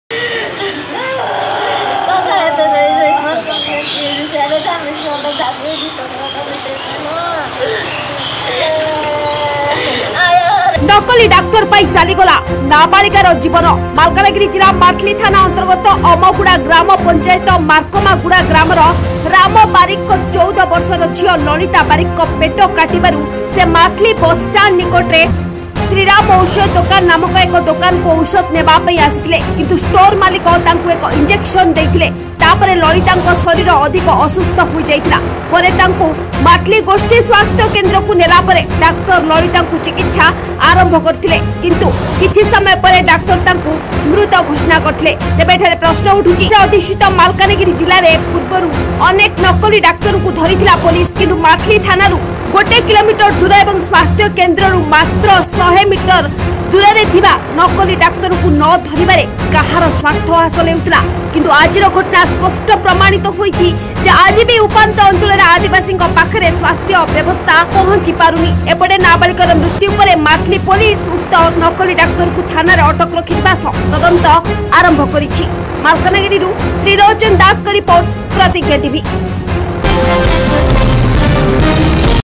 ଷ୍ଟୋର ମାଲିକ ଦେଉଛି ଇଞ୍ଜେକ୍ସନ , ଆଦିବାସୀ ଲୋକ ଜାଣିପାରୁନାହାନ୍ତି ପ୍ରକୃତ ଡାକ୍ତର କିଏ
ନକଲି ଡାକ୍ତର ପାଇଁ ଚାଲିଗଲା ନାବାଳିକାର ଜୀବନ! ମାଲକାନଗିରି ଜିଲ୍ଲା ମାଥିଲି ଥାନା ଅନ୍ତର୍ଗତ    ଅମଗୁଡା ଗ୍ରାମ ପଞ୍ଚାୟତ ମାରକାମଗୁଡା ଗ୍ରାମର ରାମ ବାରିକଙ୍କ 14 ବର୍ଷର ଝିଅ ଲଳିତା